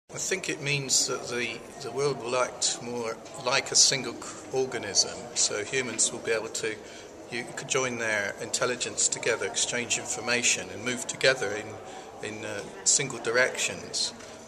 Global IGF 2010: Dave Faulkner
Dave Faulkner, director of Climate Associates LTD, UK, talks about what the future of the internet means, in ten seconds.